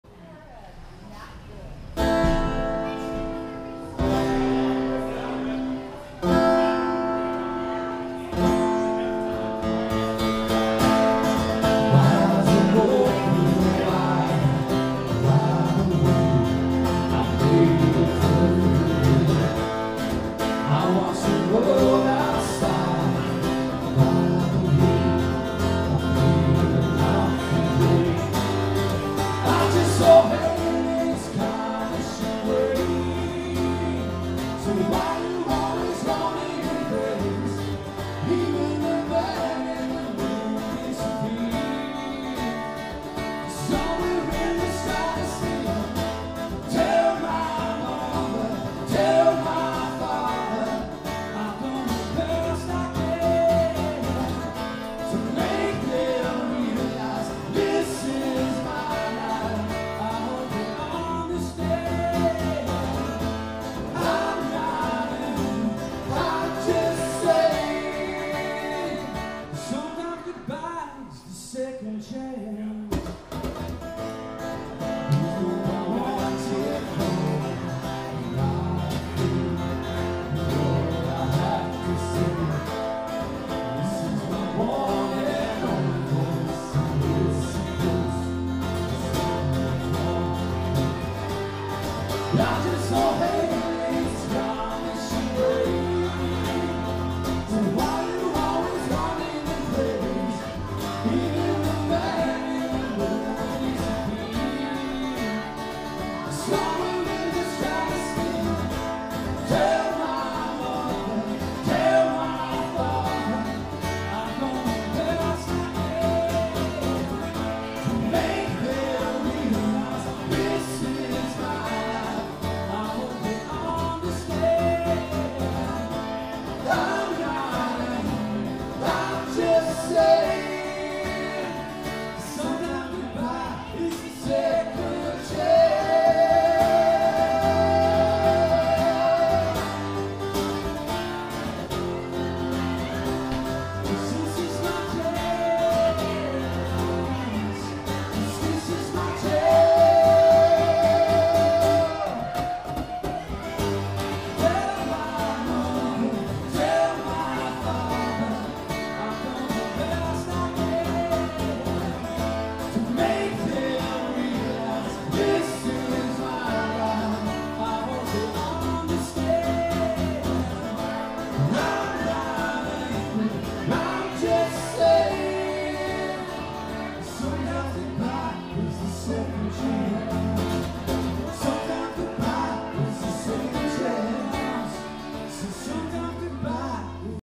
Second Chance.MOV
Simple playing at the Fish Bowl in Put in Bay August 26, 2012.